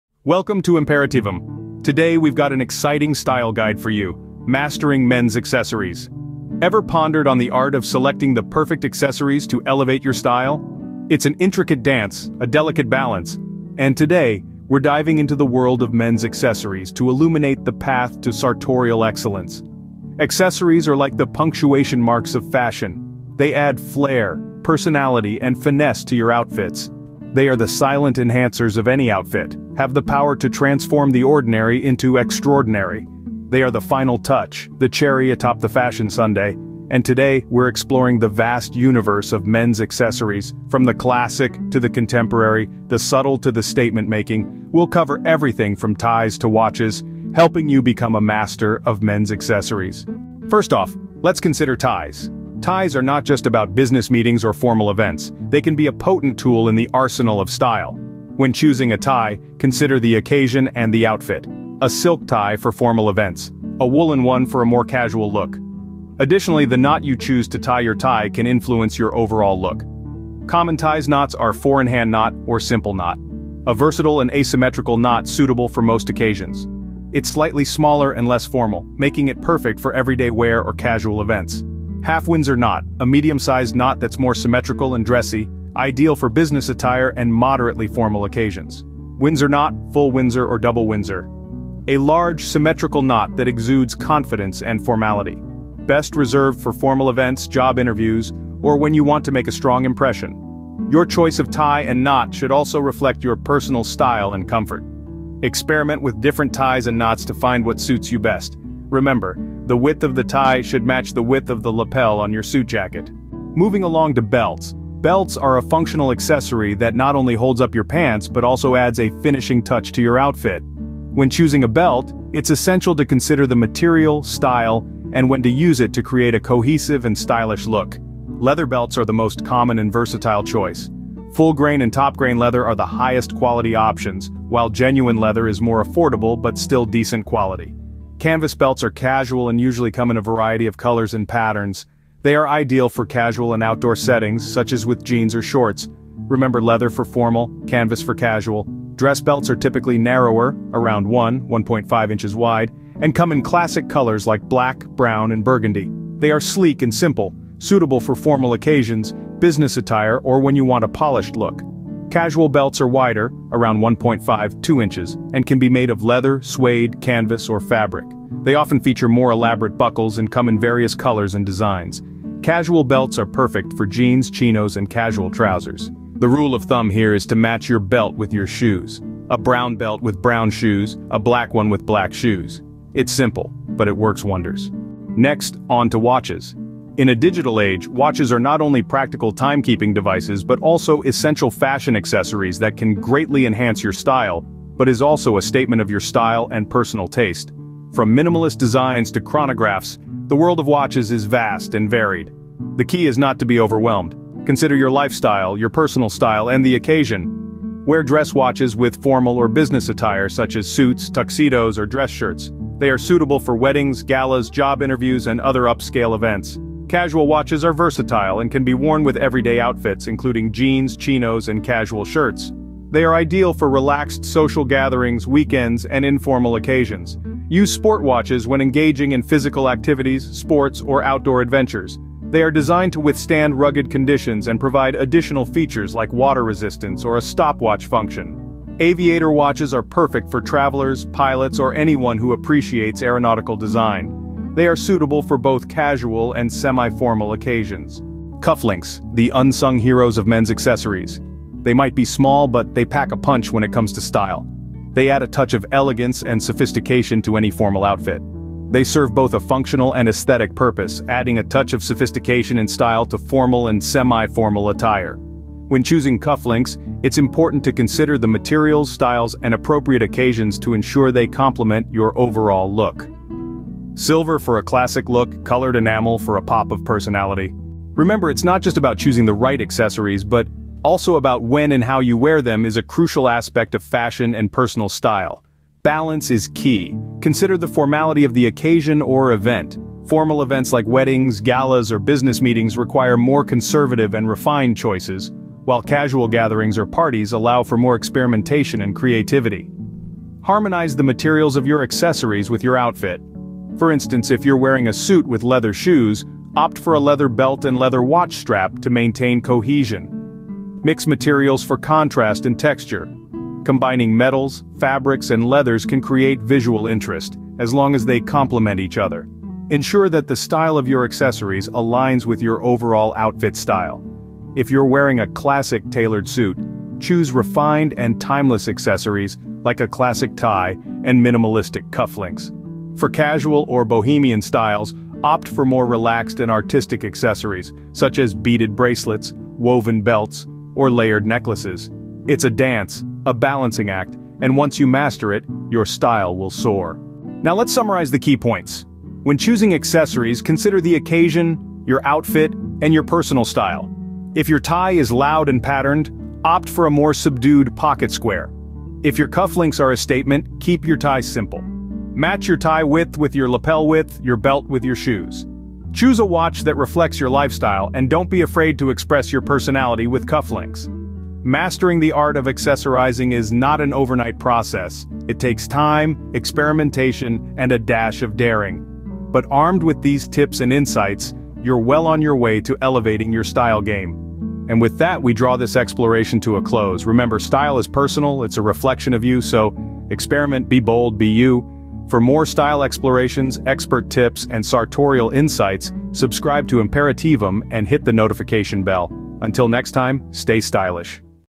Mastering Men's Accessories
Serving as the ultimate guide to buying accessories for men. Explore a wide range of accessories, including ties, belts, watches, cufflinks, and more. Provide expert insights, style tips, and practical recommendations to help viewers make informed choices when selecting accessories to enhance their outfits and refine their personal style. Cover topics such as matching accessories to different occasions, coordinating colors, and balancing the overall look. Equip your audience with the knowledge to elevate their fashion game with the perfect accessories.